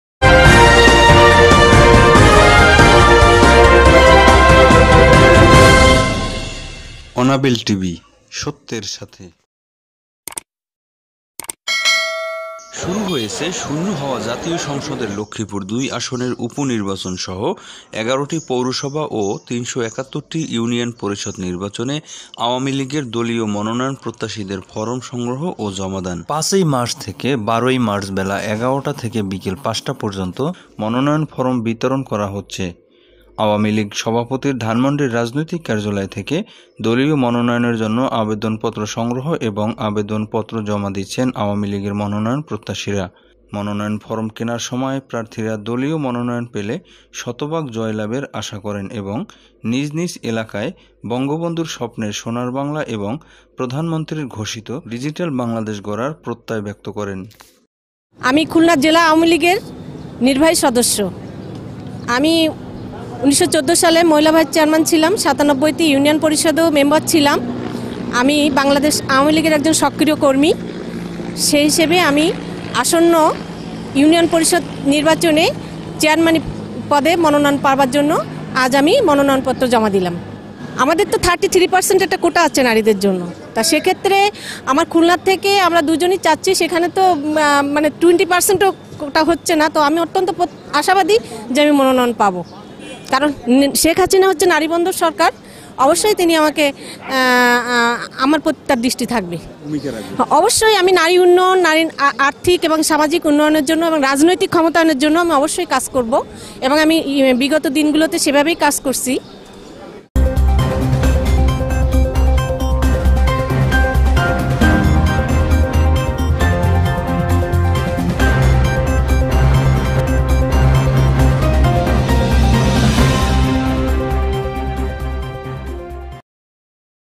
সংরক্ষিত কোঠায় চেয়ারম্যান পদে নৌকা প্রতীকে মনোনয়ন প্রত্যাশী খুলনা জেলা আওয়ামীলীগের নির্বাহী সদস্য
Anabil tv/অনাবিল টিভি